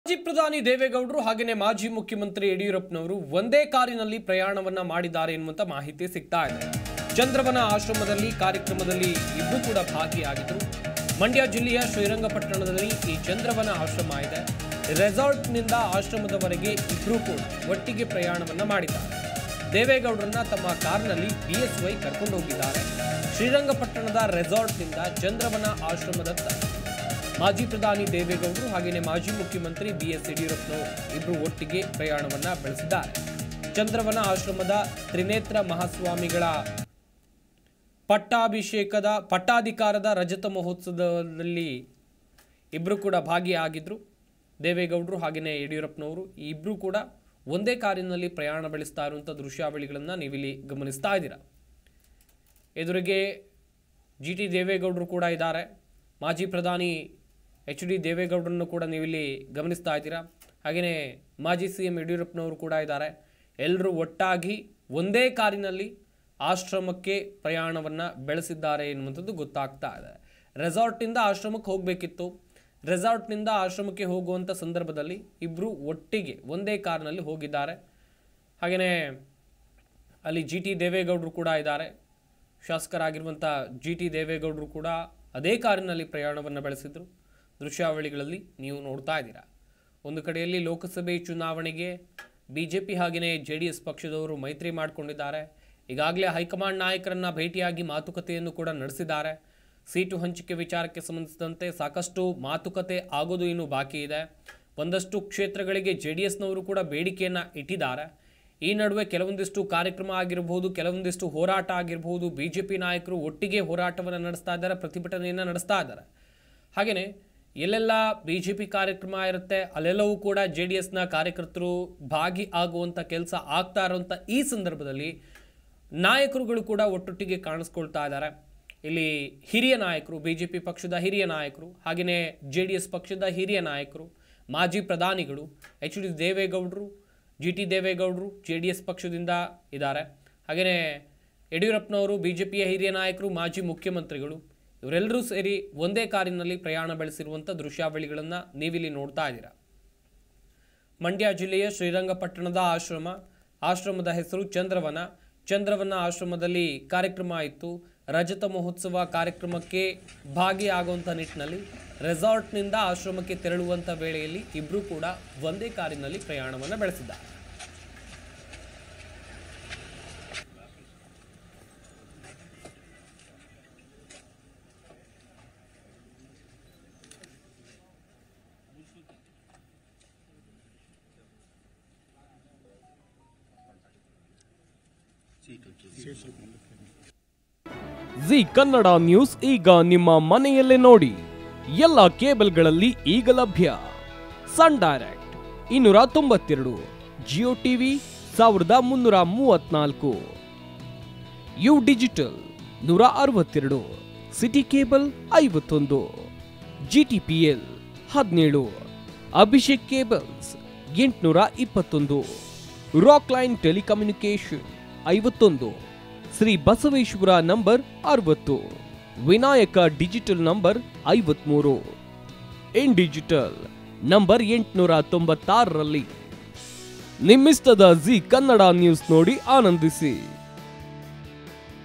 BS Yadiyurappa and HD Devegowda | ಚಂದ್ರವನ ಆಶ್ರಮದಲ್ಲಿ ಕಾರ್ಯಕ್ರಮದಲ್ಲಿ ಭಾಗಿ
ಮಂಡ್ಯ ಜಿಲ್ಲೆಯ ಶ್ರೀರಂಗಪಟ್ಟಣದ ಆಶ್ರಮ
ರೆಸಾರ್ಟ್‌ನಿಂದ ಆಶ್ರಮದವರೆಗೆ ಒಟ್ಟಿಗೆ ಪ್ರಯಾಣ
ದೇವೇಗೌಡರನ್ನು ತಮ್ಮ ಕಾರಿನಲ್ಲಿ ಕರೆದೊಯ್ದ BSY
ಶ್ರೀರಂಗಪಟ್ಟಣದ ರೆಸಾರ್ಟ್‌ನಿಂದ ಚಂದ್ರವನ ಆಶ್ರಮ